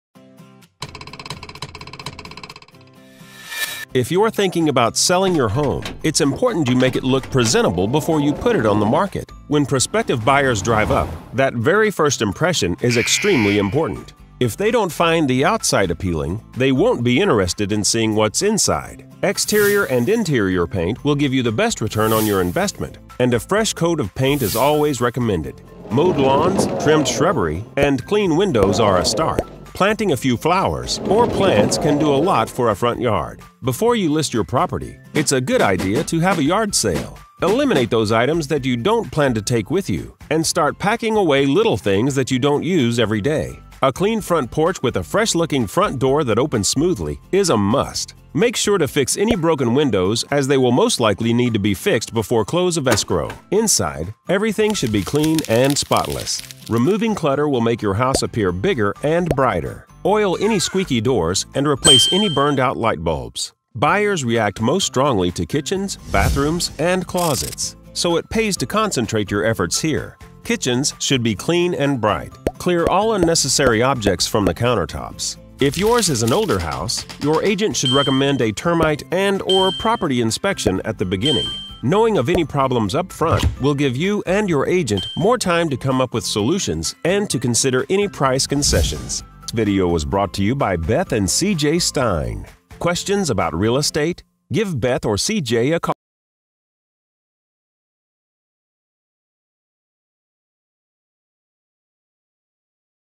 Get Your Home Ready To Sell
Stein Realty Group provides a quick overview on how to get your home sale ready.